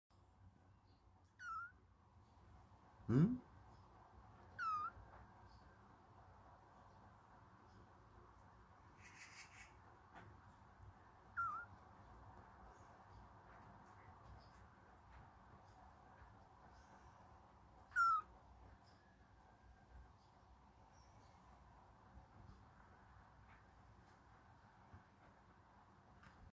고양이 구조
비 맞고 움직이지 못하는 고양이를 데려왔어요